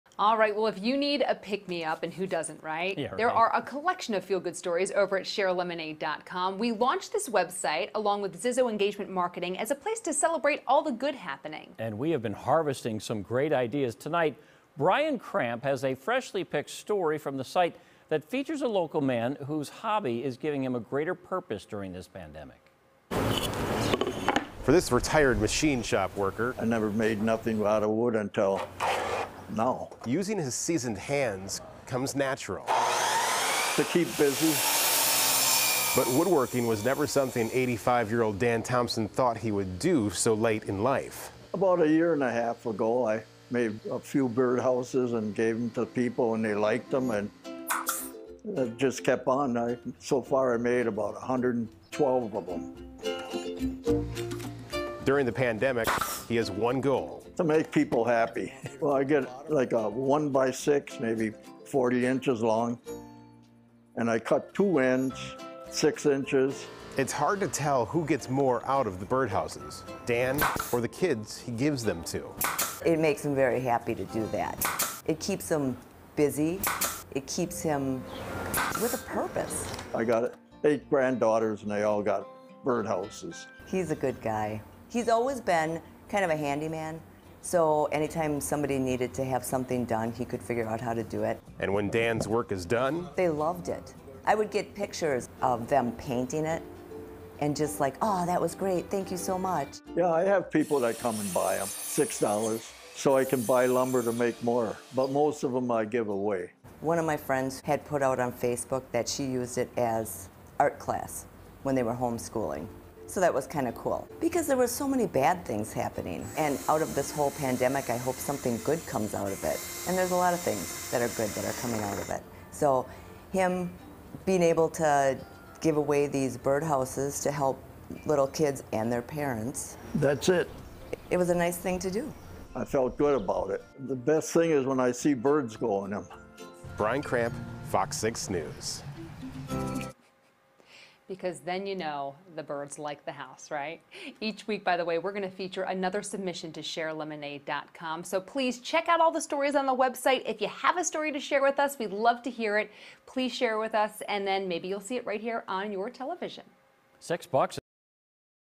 Hartford man builds birdhouses in effort to make people happy amid pandemic
If you need a pick-me-up, there are a collection of feel-good stories at ShareLemonade.com.

It's a place to celebrate the good.

One freshly-picked story from the site features a birdhouse builder on a mission.